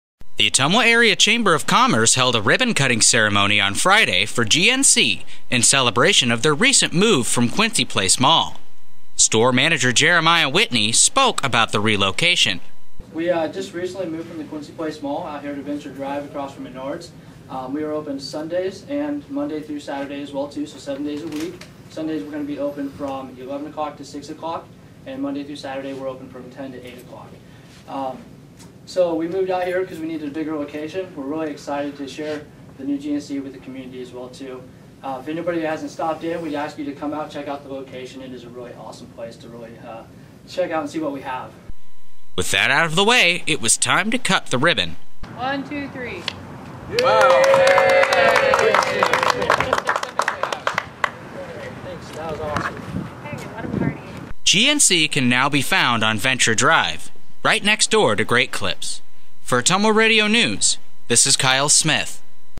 GNC Ribbon Cutting
The Ottumwa Area Chamber of Commerce held a ribbon cutting ceremony for GNC on Friday.